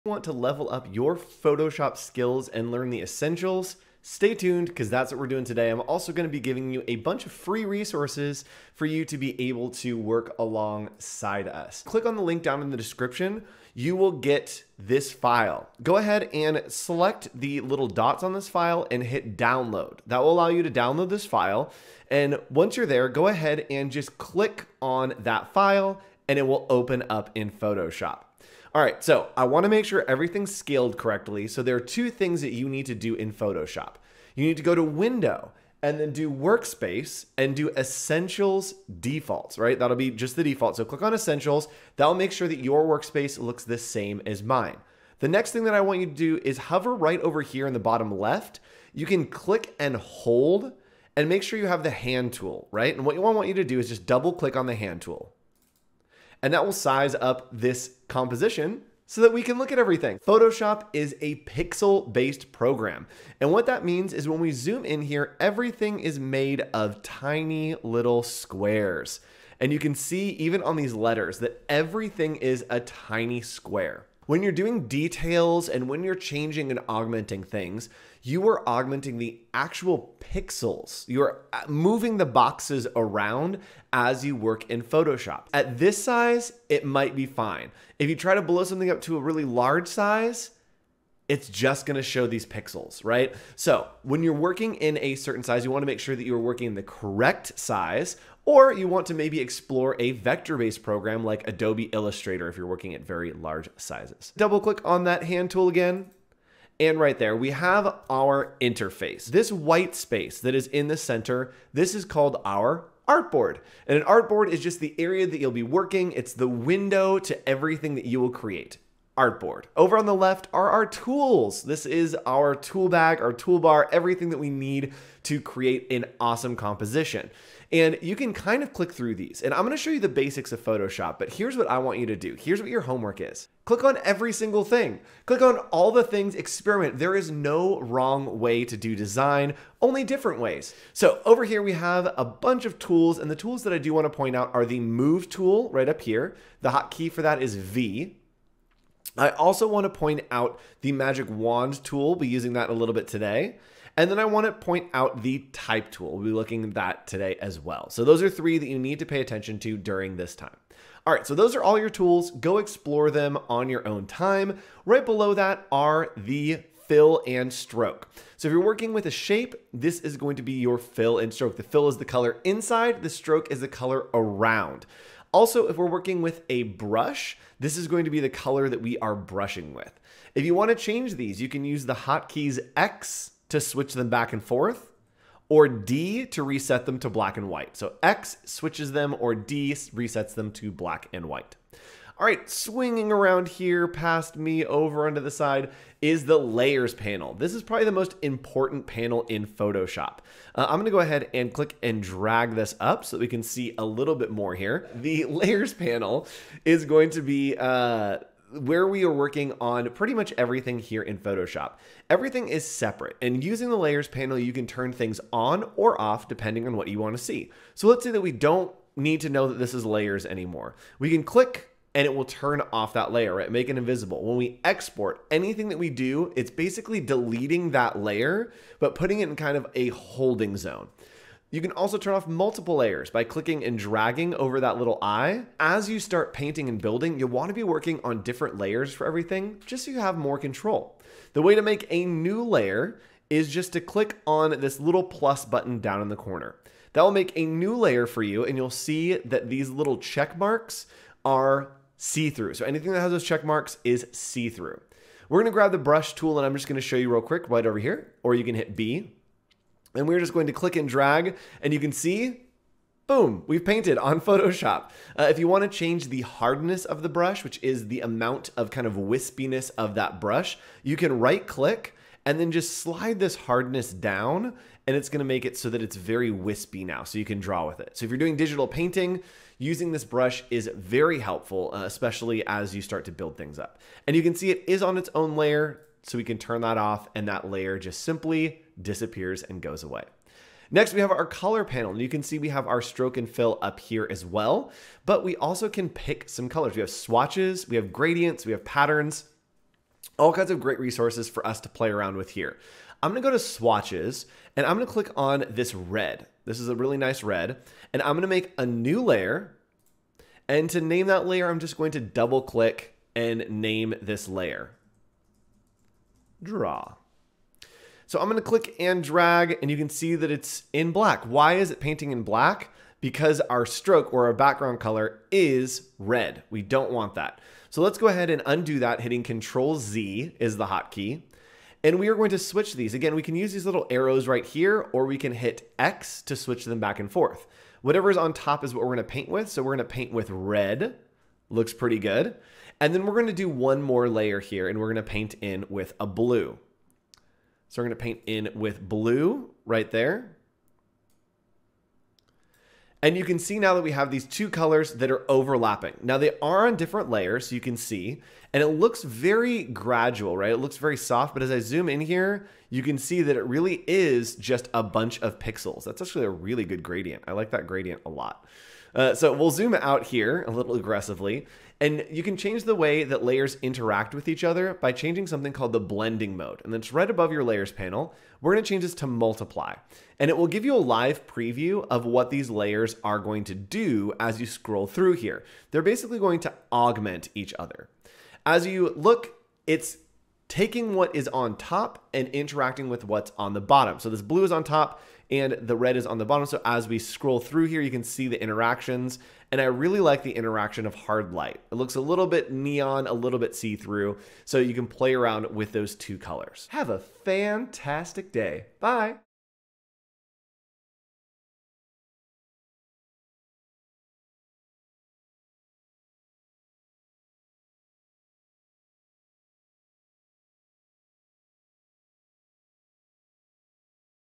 How to Use Photoshop For the First Time | Tutorial for Beginners | Adobe Photoshop
New to Photoshop? We've got you. Learn the Photoshop essentials and explore the Photoshop toolbar in this tutorial for beginners. You'll learn how to use the Brush Tool, create your own paintings, and more.